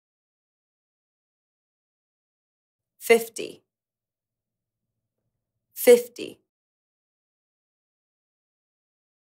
How to pronounce FIFTY in American English
This video shows you how to pronounce FIFTY in American English. Speaker has an accent from New York City, NY. Collins Dictionary, the home of living English and pioneers of dictionary publishing